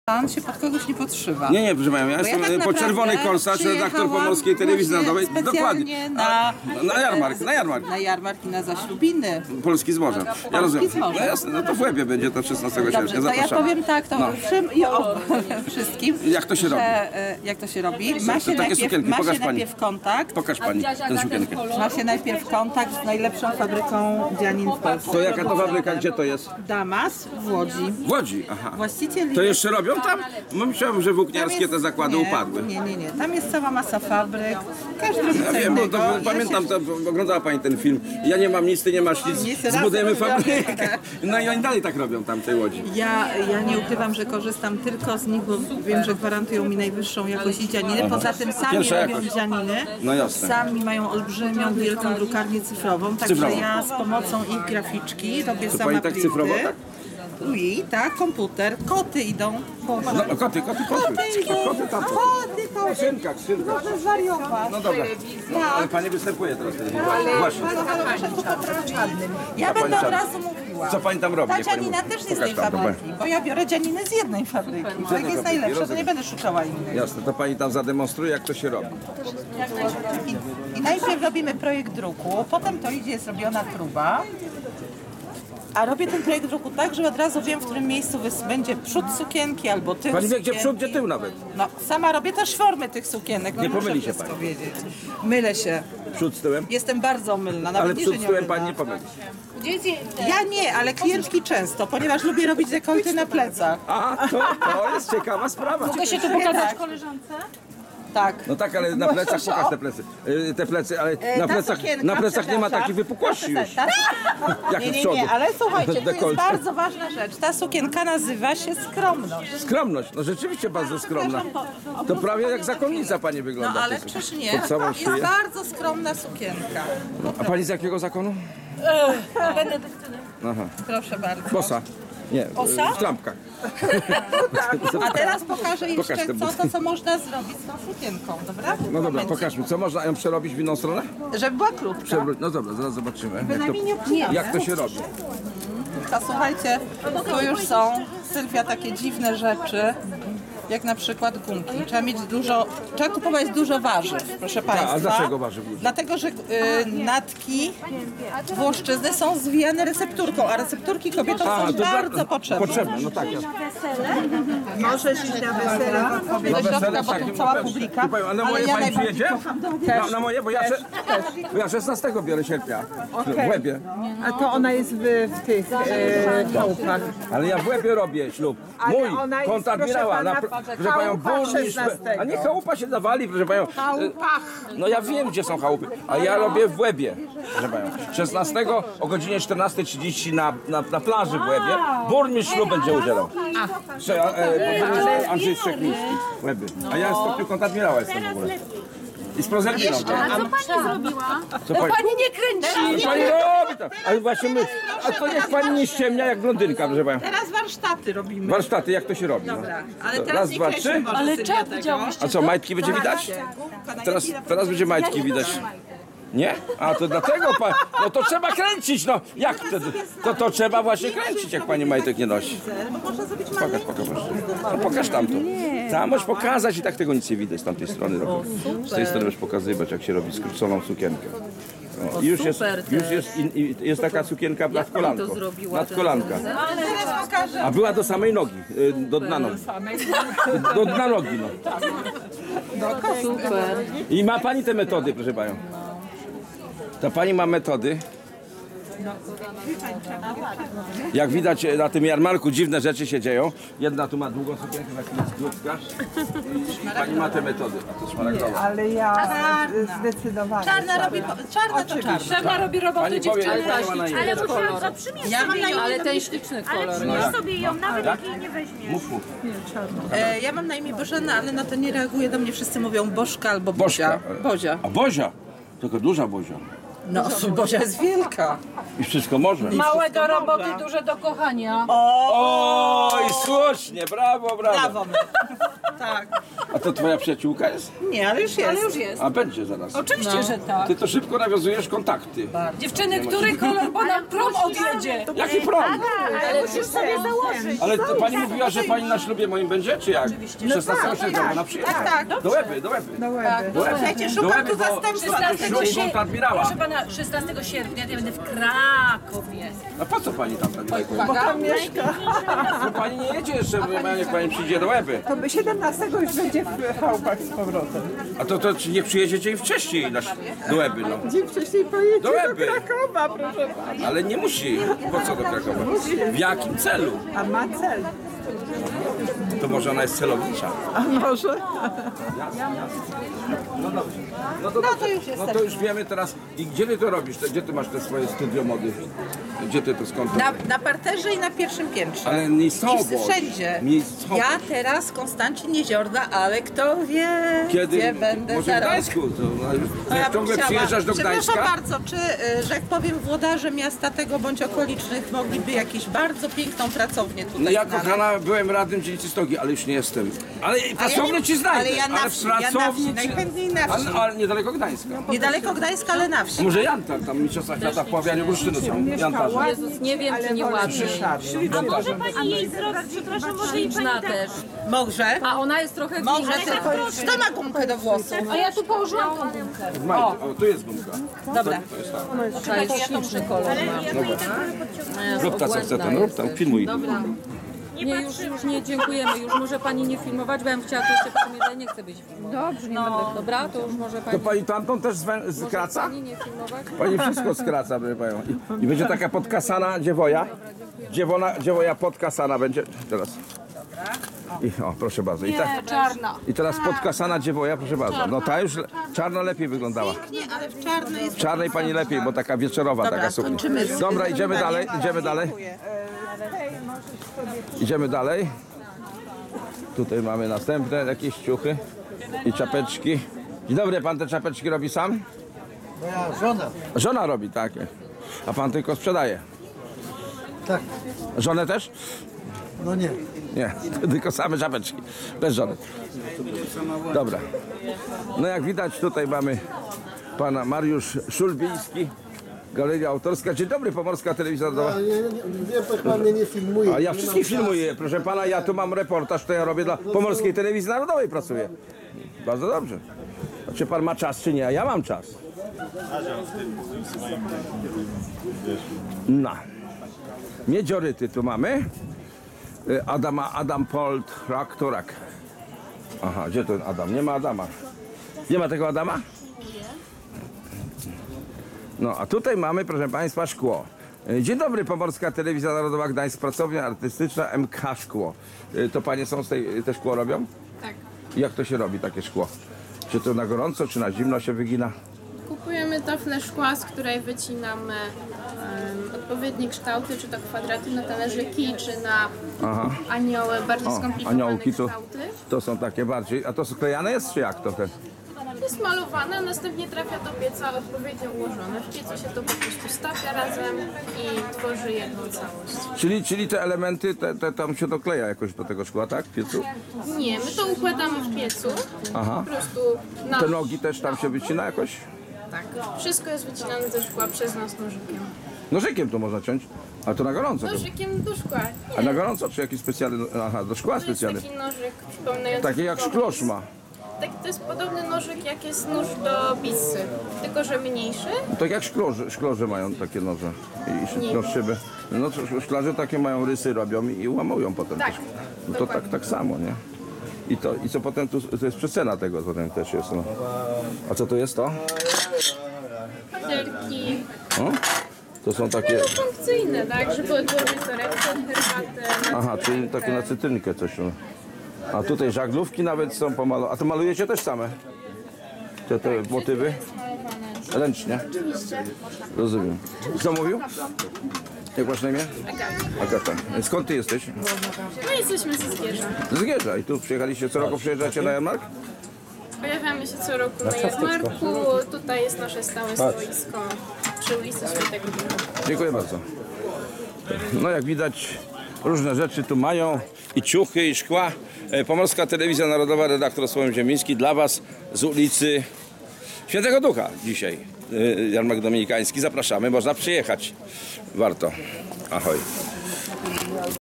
CZERWONY KORSARZ , BOŻENA KARSKA I JEJ KREACJE PREZENTOWANE NA JARMARKU ŚWIĘTEGO DOMINIKA 2019r.
Na Jarmarku świętego Dominka w Gdańsku jest wielu artystów prezentujących i sprzedajacych swoje wyroby.Jedni tworzą w bursztynie inni w skórze lub w szkle ale są i tacy jak pani Bozena Karska ,  która jest artystką Jej piękne suknie ,  które to chętnie przymierzaja i kupują ładne panie są naprawde unikatowe.
Warto przyjechać do Gdańska na Jarmark,   bo tu kazdy znajdzie coś dla siebie!